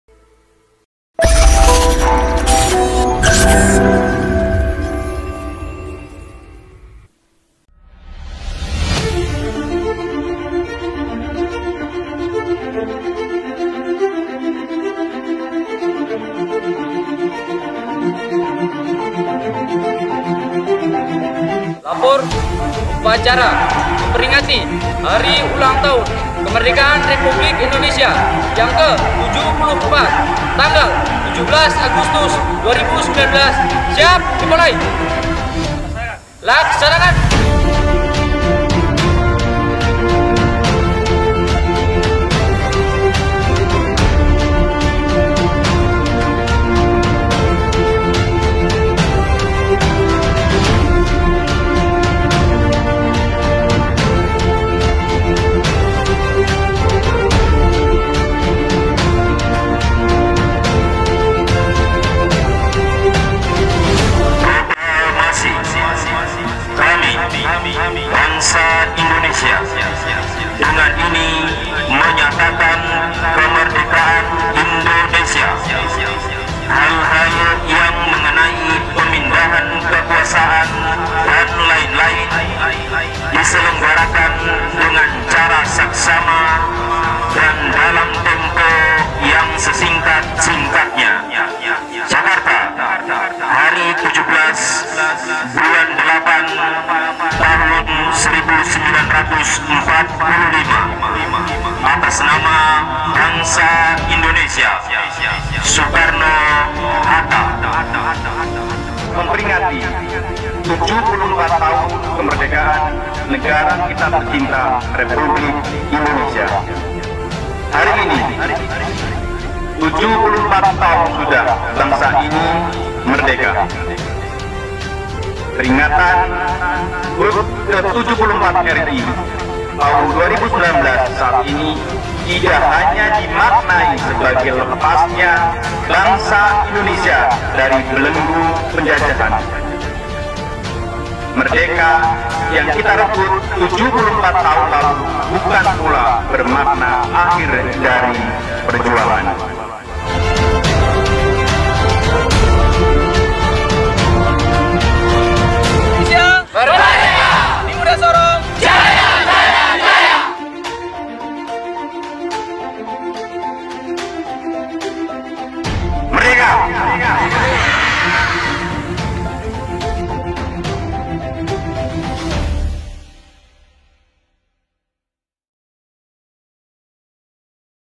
HUT ke-74 Kemerdekaan Republik Indonesia
Lapangan Hijau Khalid bin Walid kampus UNIMUDA Sorong di hari Sabtu pagi (17/8/19) mendadak berubah warna menjadi “merah dan Putih” oleh para peserta upacara bendera dalam rangka memperingati Hari Ulang Tahun (HUT) ke-74 Kemerdekaan Republik Indonesia (RI).

Upacara mulai berlangsung pukul 07.30 WIT, diikuti ribuan peserta dengan Inspektur Upacara (Irup) Wakil Rektor UNIMUDA Sorong, Doni Sudibyo, M.Pd., dan Komandan upacara Septian Pramono (Mahasiswa FKIP-Program Studi Pendidikan Jasmani).

Hee-hee-heey! Selamat datang di channel UNIMUDA SORONG
Kalian bisa check social media  di bawah ini :
FansFacebook jas merah creative     :

mediaunimuda